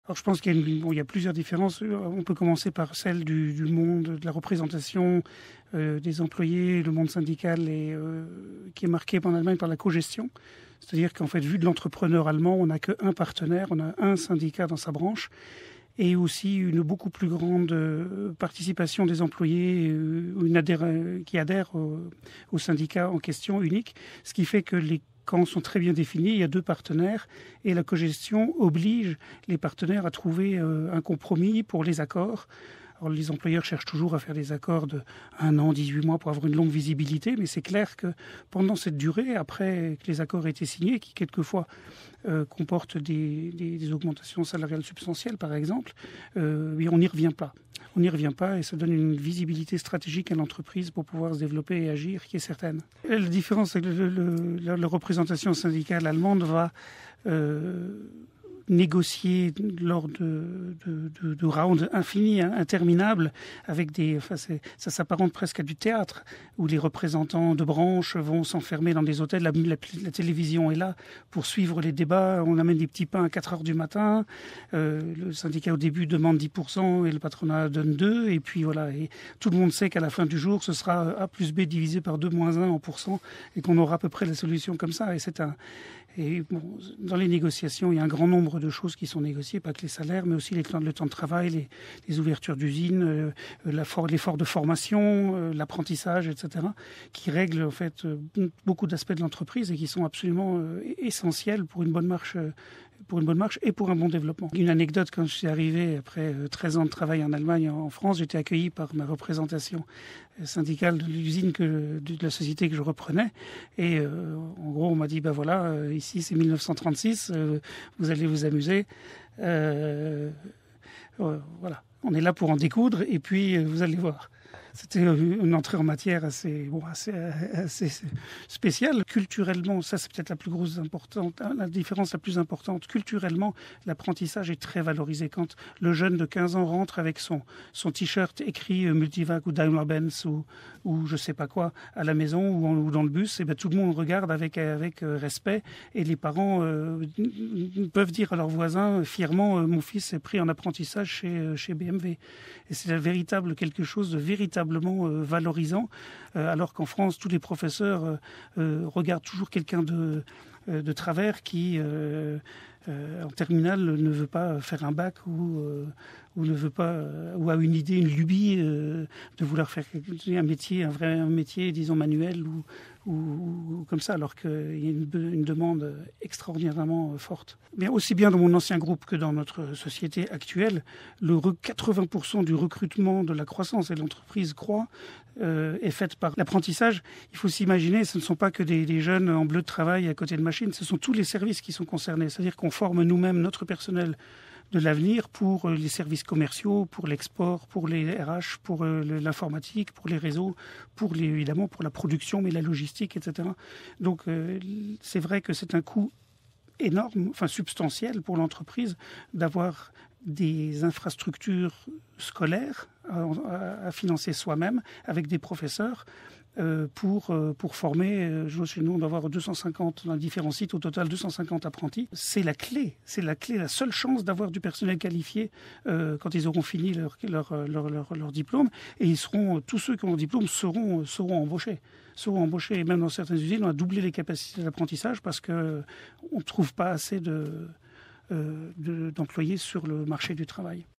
"Culturellement, l'apprentissage est très valorisé en Allemagne, contrairement à la France."
Christophe Charoy est président de Multivac, une société allemande basée en France. L’entreprise travaille dans la maintenance de machines agroalimentaires. Il a travaillé 13 ans en outre-Rhin et répond à François-Pierre Noël.